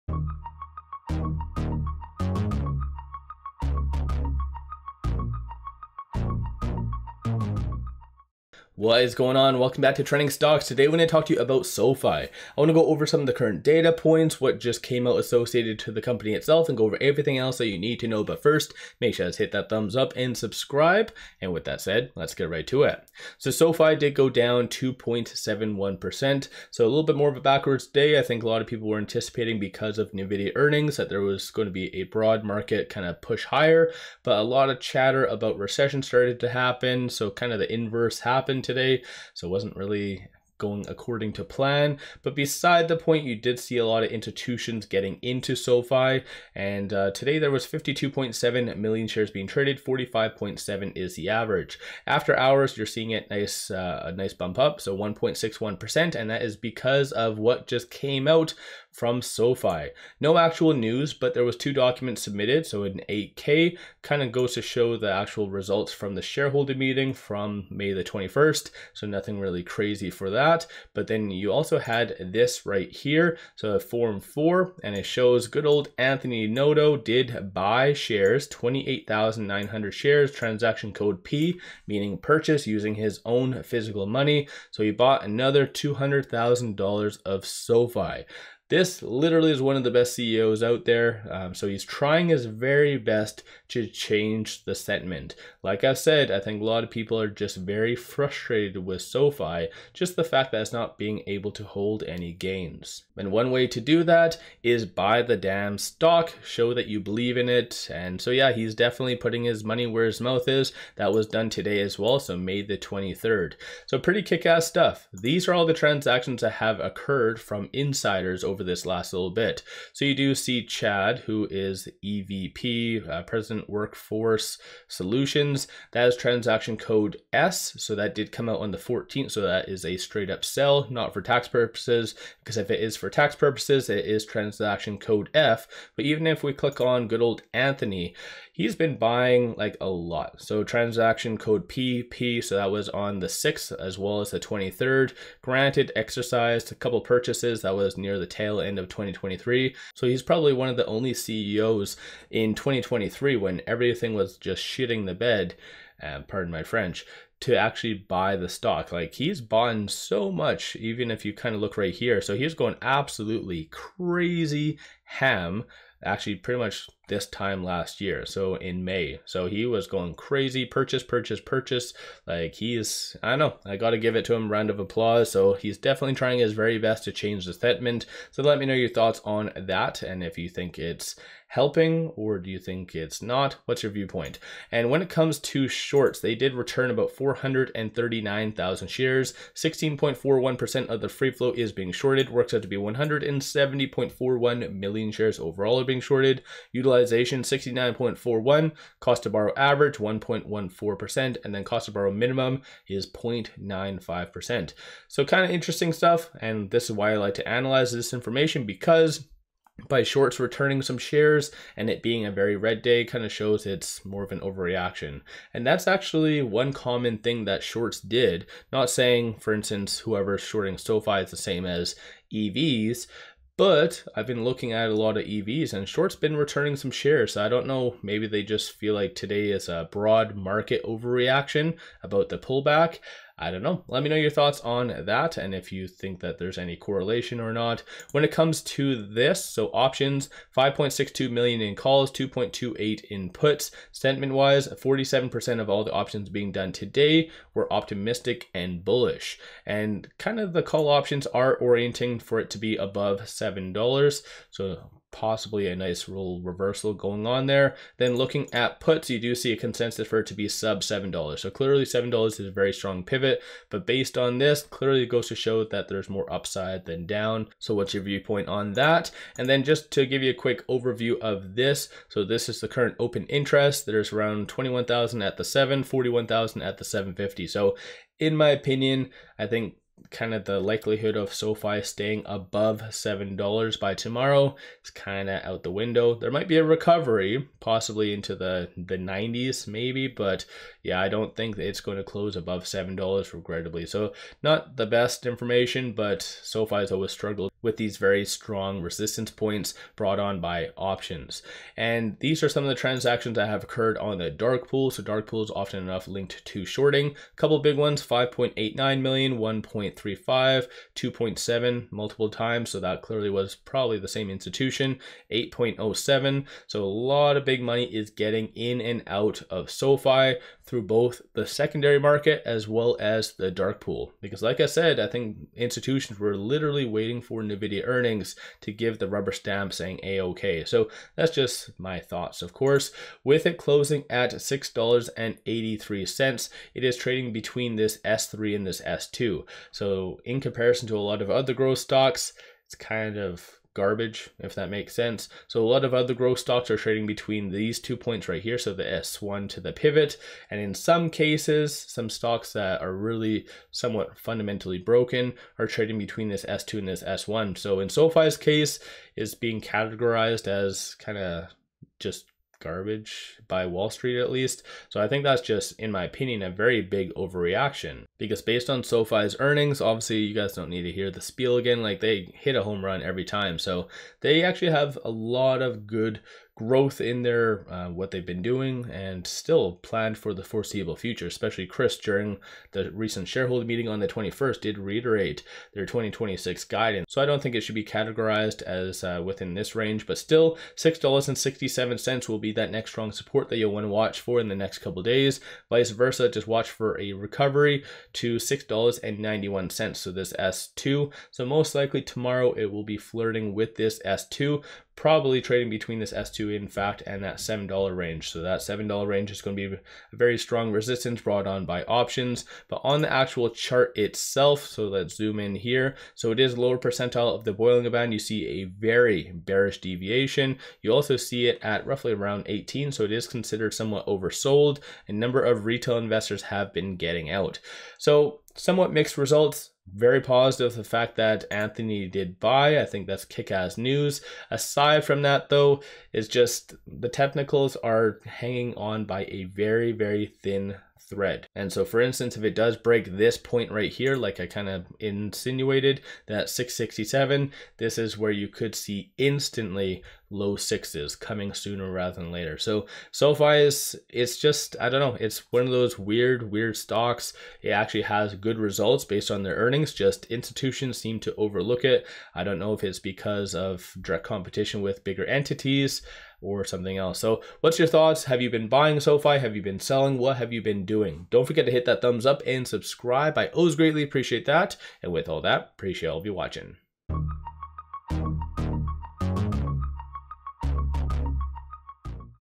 NOTO Buys $200K in SoFi TODAY │ What is Coming NEXT for SoFi ⚠️ SoFi Stock Analysis
Vid on what happened with SoFi today. sofi sofistock $SOFI